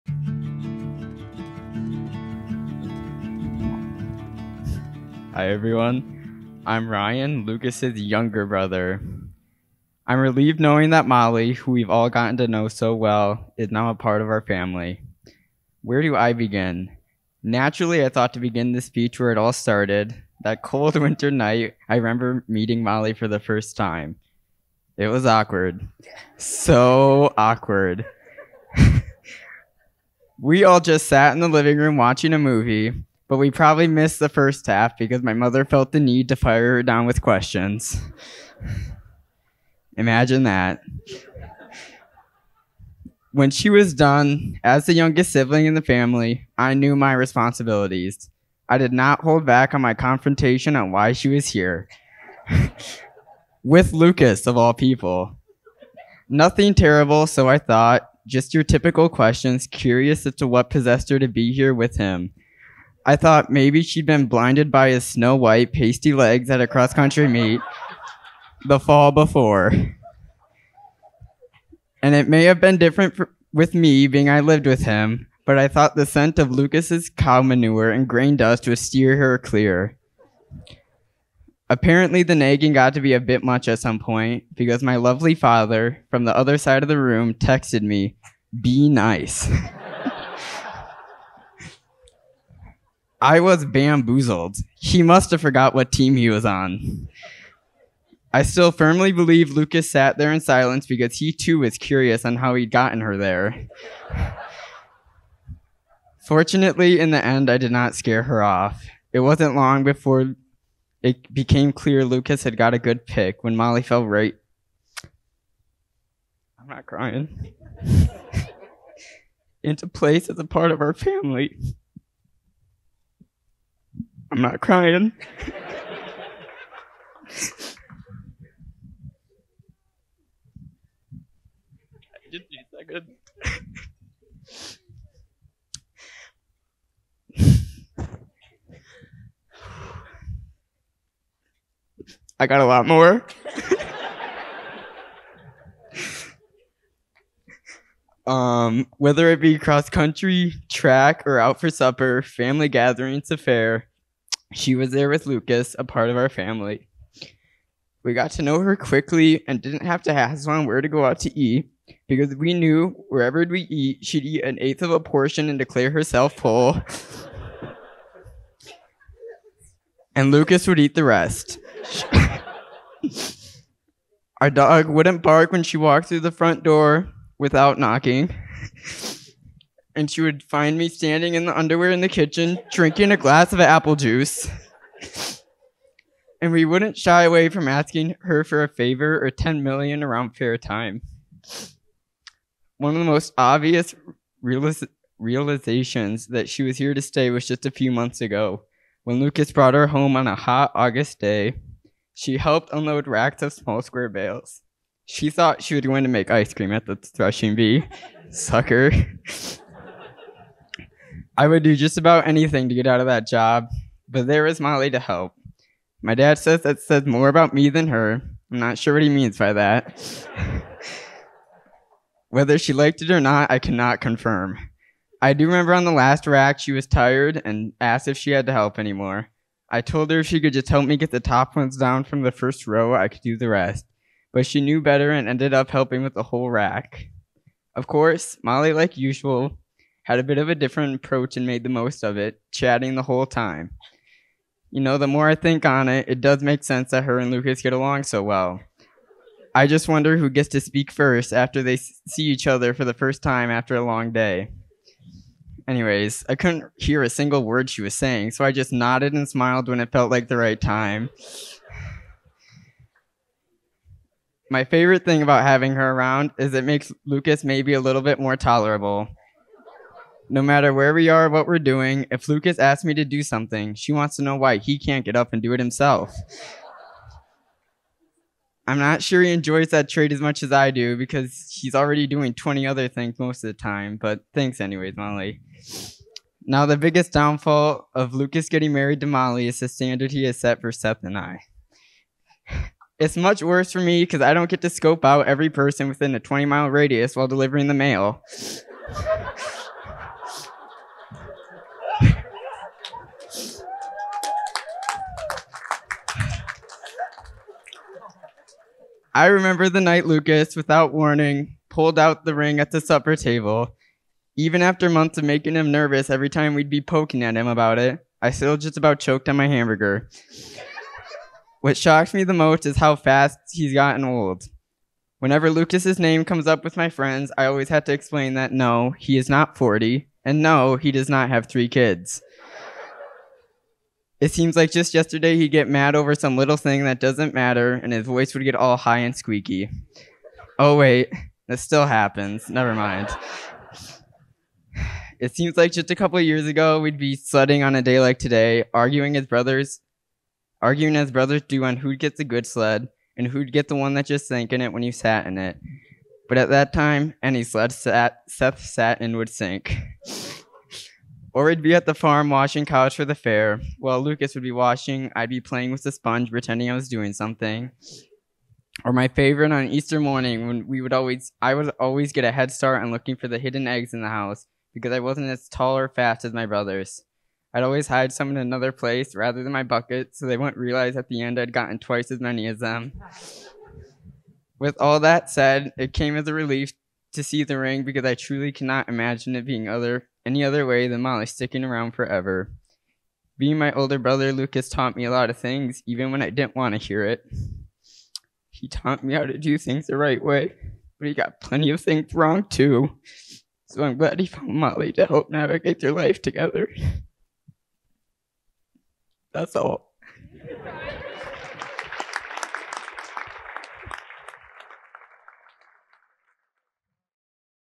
I'm Not Crying | Little Brother Nails Best Man Speech | Full Video
I shared a YouTube Short as well as Facebook and Instagram Reels of a portion of Ryan's speech from Lukas and Molly's wedding. I shouldn't be surprised that many people found the speech as endearing, wholesome, and funny as I did. Many of you wanted to see the entire speech, so I am finally uploading the full speech from Molly and Lukas' wedding in Sleepy Eye, MN.

I will share more speech videos like this in the future as well.